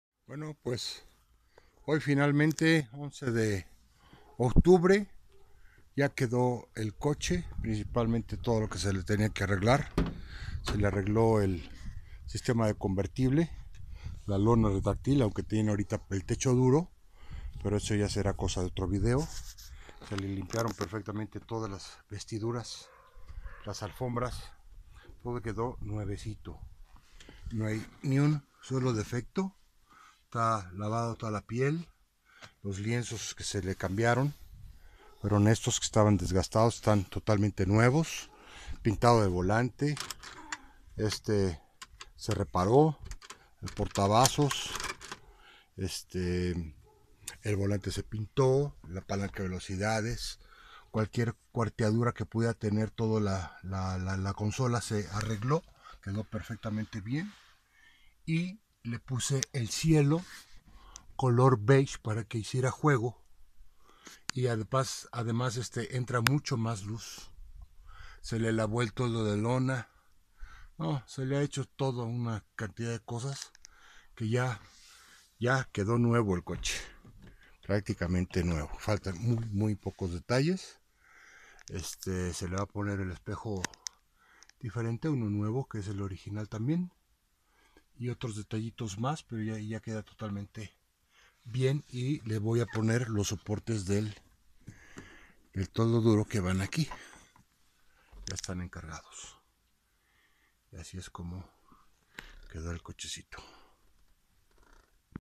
BMW Z4 2004 2.5I Impecable II
Estado de conservación de un BMW Z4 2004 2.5i